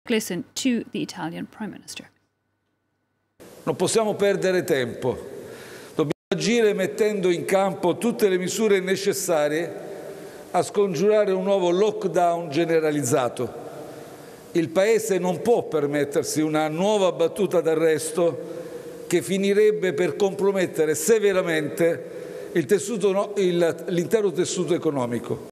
Coronavirus pandemic: Italian PM announces stricter measures to counter virus
Italian Prime Minister Giuseppe Conte on Sunday announced a new series of measures to tackle the rising number of virus cases, with restrictions placed on bars and restaurants and push to increase working from home.